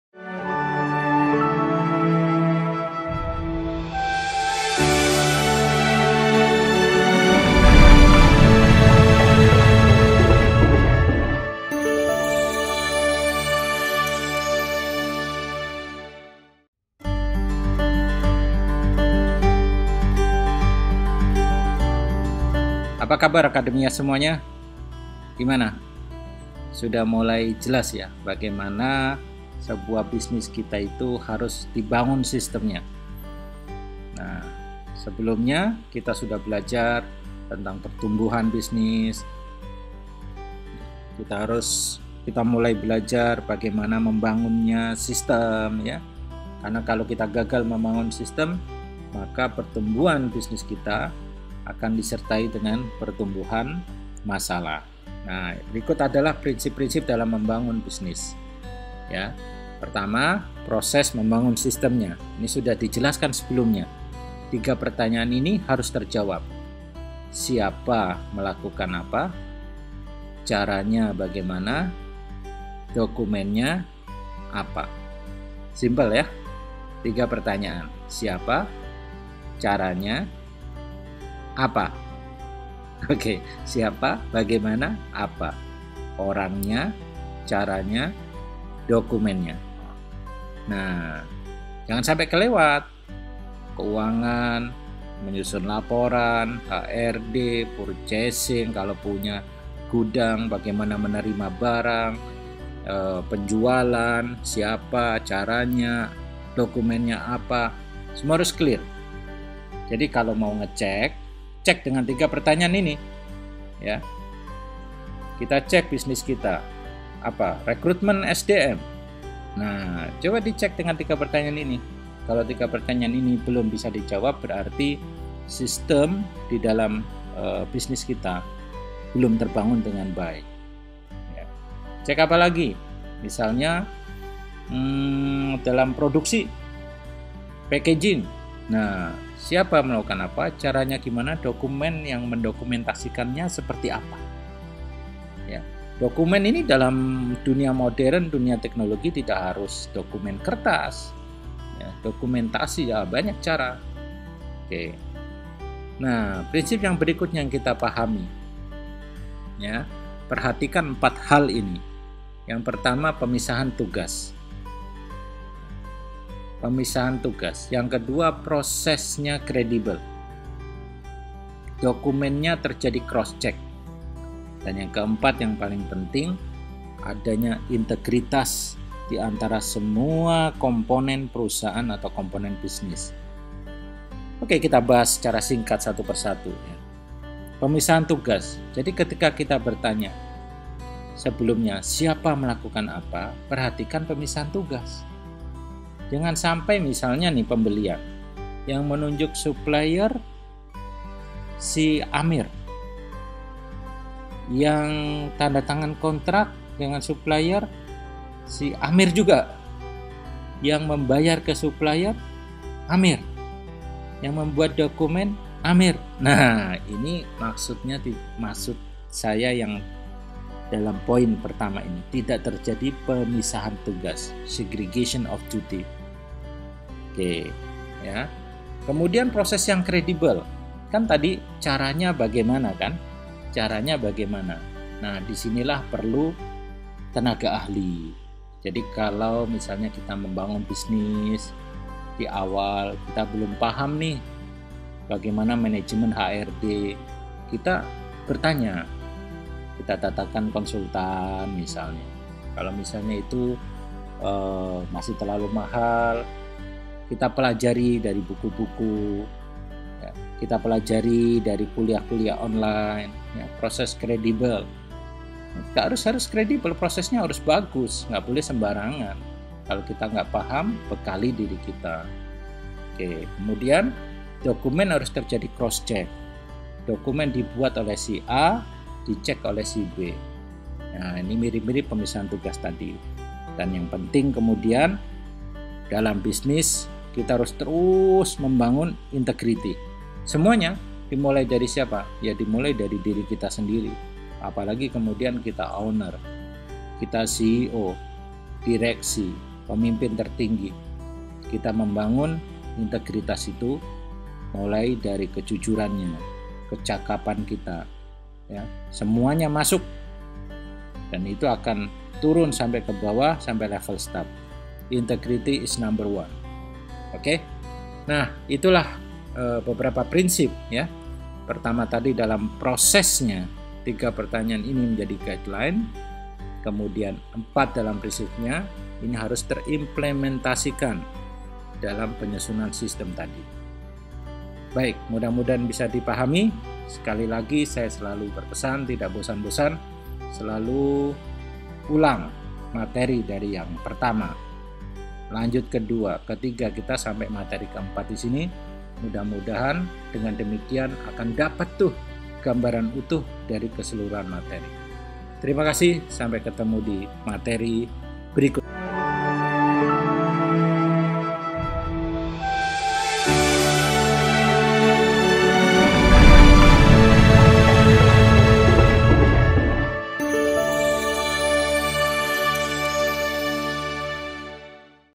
[Membangun Sistem Bisnis] 4 Prinsip Membangun Sistem
Kita memerlukan sistem dalam bisnis kita. Sistem yang membuat bisnis berjalan "auto pilot", sehingga semua business process berjalan lancar tanpa harus selalu kehadiran pelakunya.